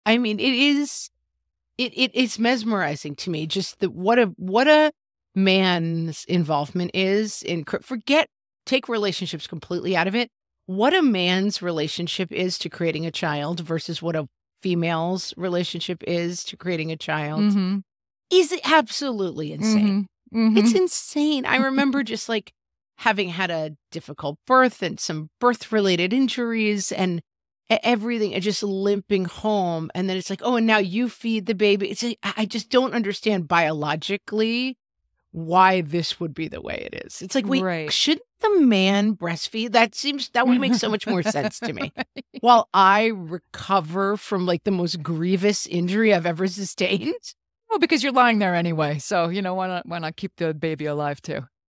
Shouldn't men breastfeed?
After birth, the birthing parent's life is completely altered, and their partner's life is...well, mostly the same.